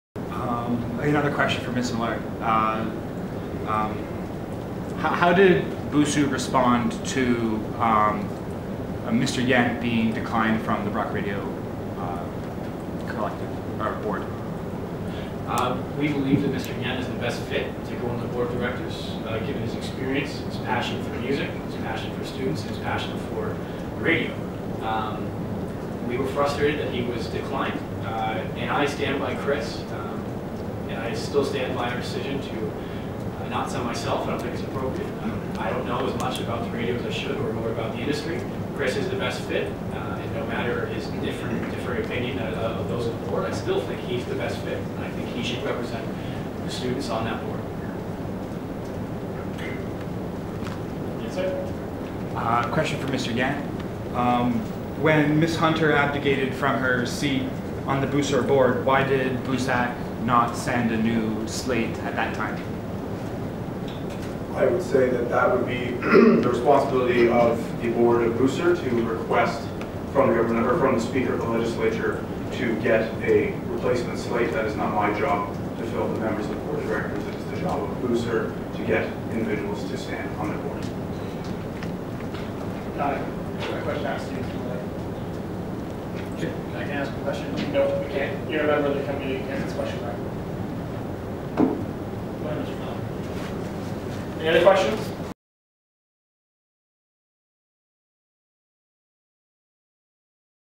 How did BUSU respond to BUSR declining Chris Yendt? BUSAC Nov 5, 2013
BUSU president Cooper Millard, and BUSU Vice President of Finance and Administration, respond to questions at Nov 5, 2013 BUSAC meeting.

Video by Daniel Nardone | BrockBUG.com